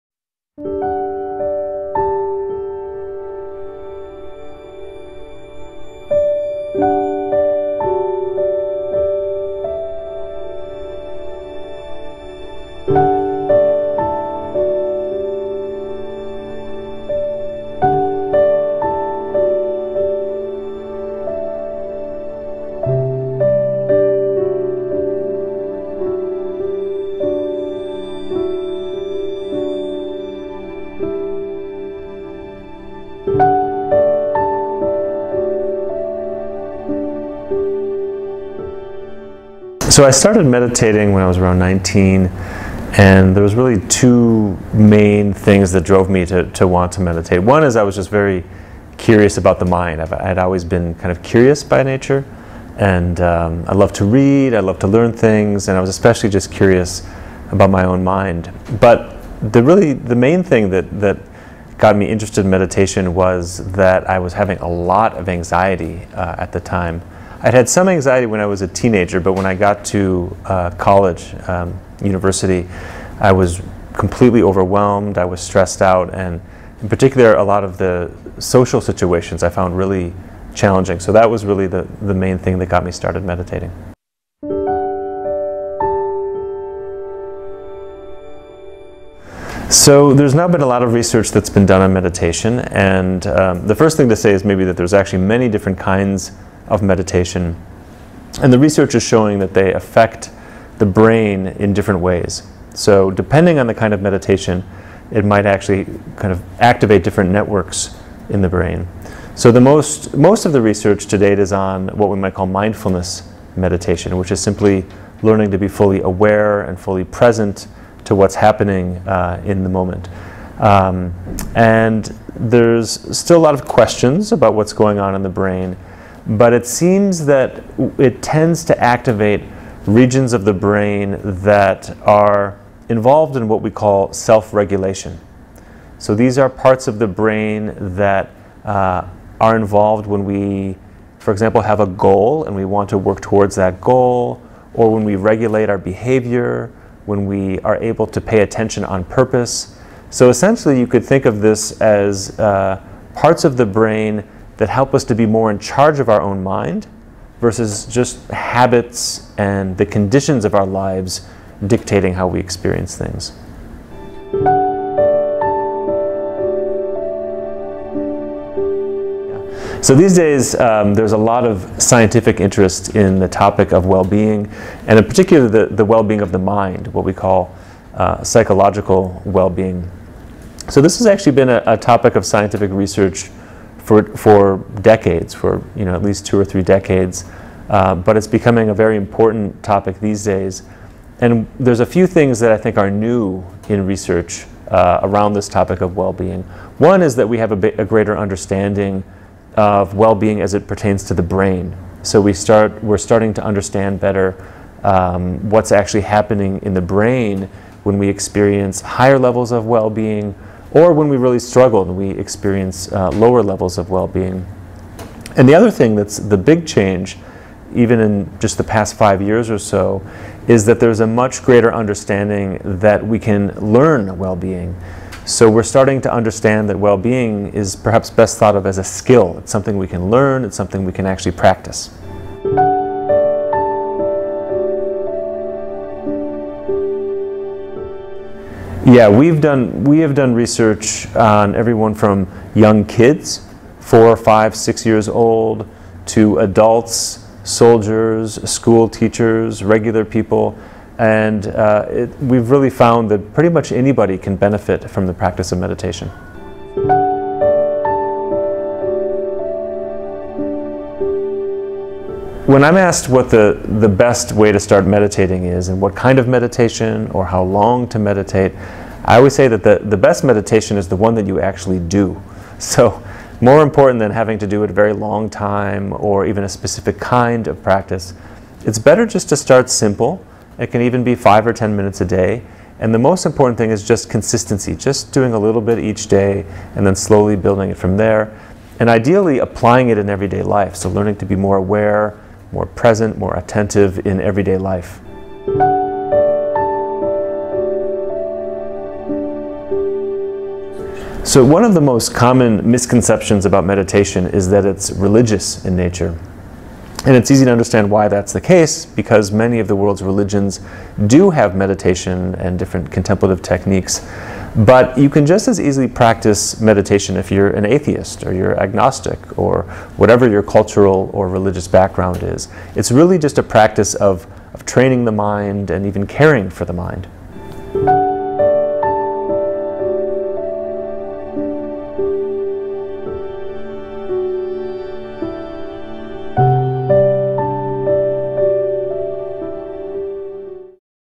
É POSSÍVEL APRENDER A SER FELIZ? A LIGAÇÃO ENTRE BEM-ESTAR PSICOLÓGICO E MEDITAÇÃO
As mais recentes pesquisas no campo das neurociências mostram que a sensação de bem-estar produzida pelo cérebro é uma habilidade que pode ser desenvolvida através da prática da meditação, afirma nesta entrevista o Dr.Cortland Dahl.

Dr.Cortland Dahl pratica a meditação há mais de vinte anos e estudou budismo no mundo todo, incluindo oito anos vivendo em assentamentos de refugiados tibetanos na Índia e no Nepal. Ele possui um Ph.D. em Mente, Cérebro e Ciência Contemplativa na Universidade de Wisconsin e mestrado em Estudos Budistas na Universidade de Naropa. Atualmente, atua como cientista pesquisador e diretor chefe do Center of Healthy Minds da Universidade do Wisconsin e presidente do Tergar International. Como autor e tradutor, publicou numerosos artigos científicos e livros sobre a prática budista.

Agradecimentos ao Tergar International.